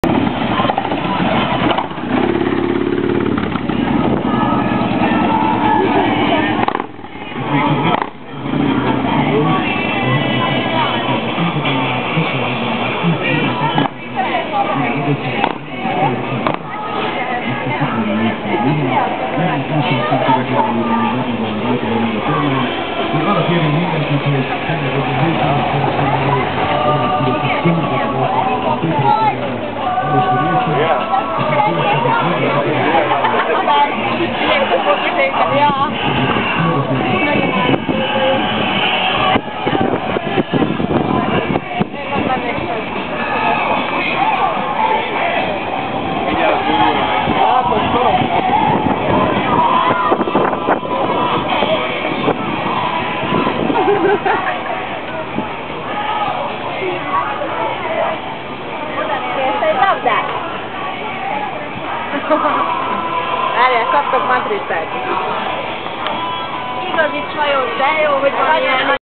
Csajok a motoron a 12. Harley-Davidson Open Road Festen
2011-ben első alkalommal vettünk részt saját standdal a 12. Harley-Davidson Open Road Fesztiválon.